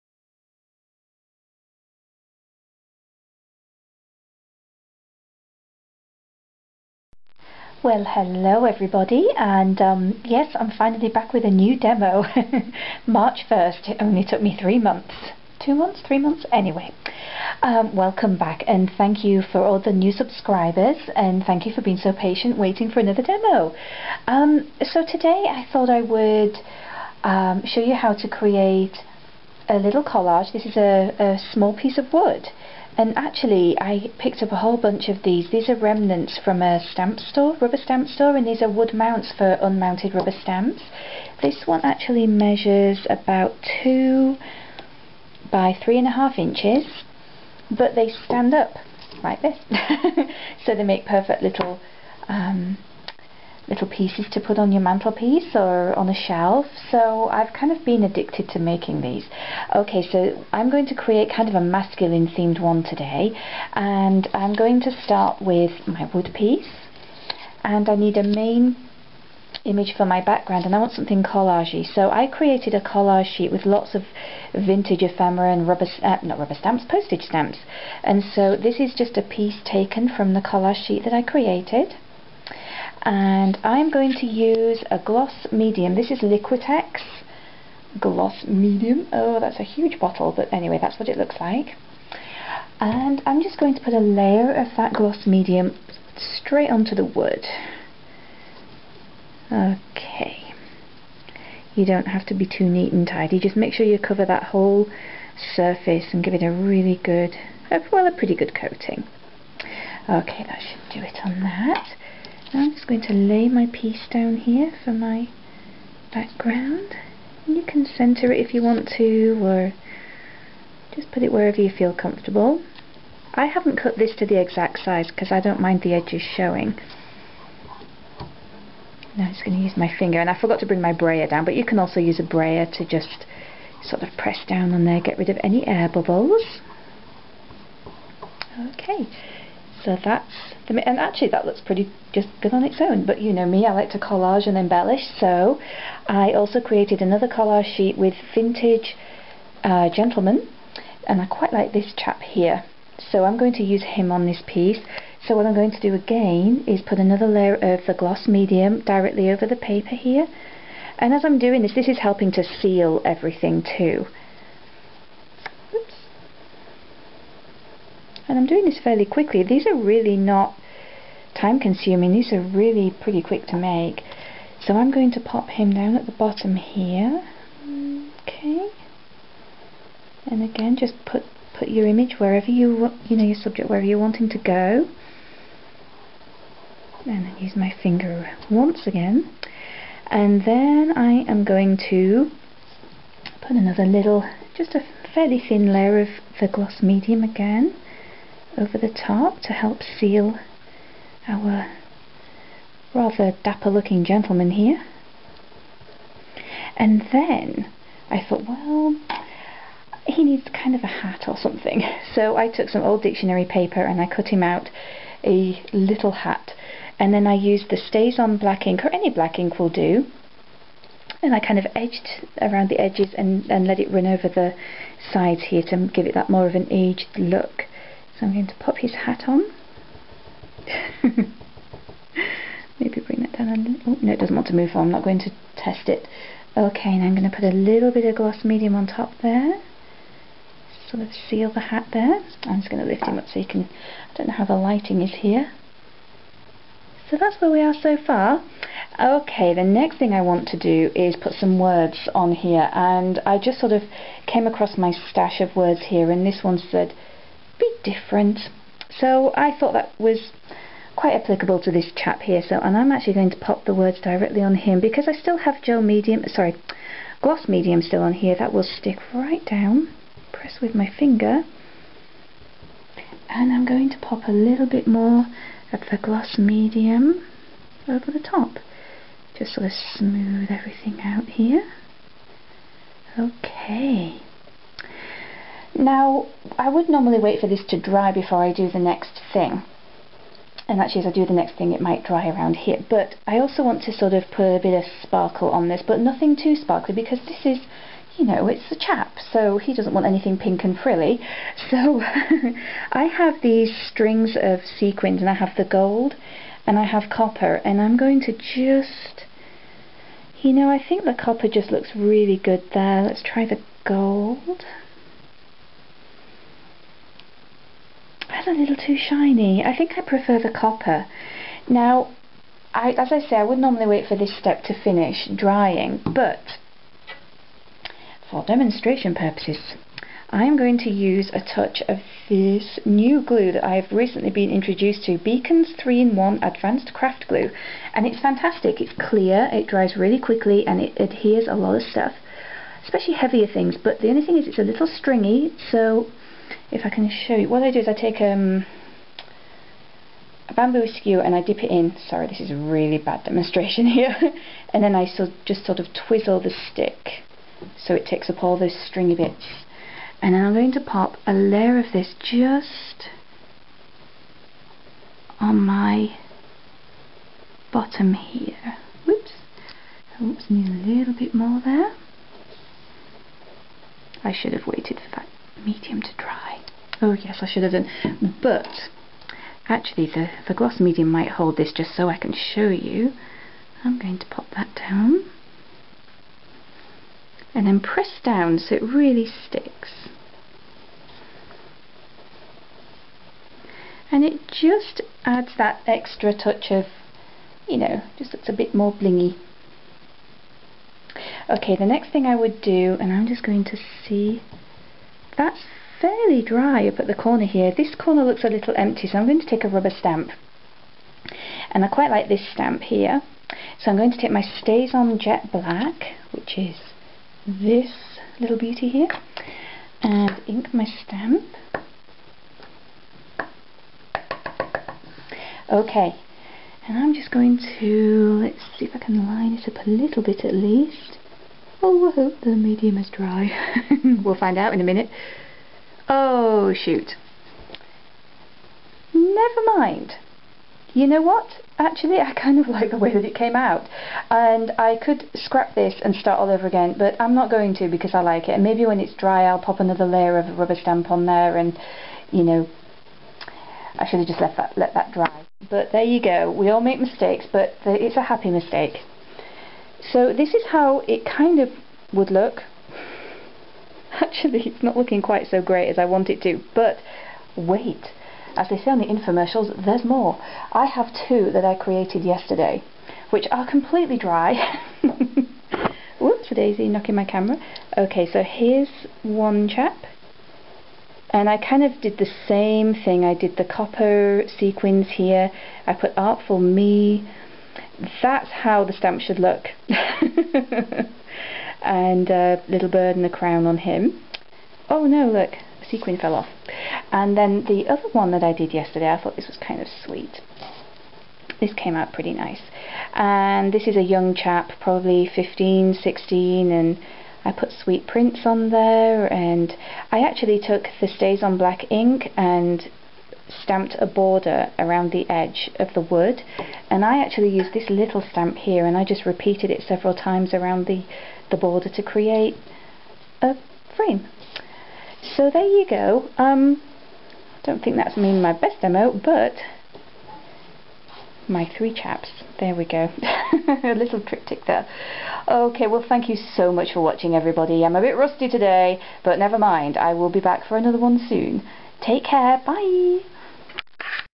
Mixed Media Collage On Wood
Learn how to create a collage on a piece of wood. I hope you enjoy this little demo - it's my latest addiction.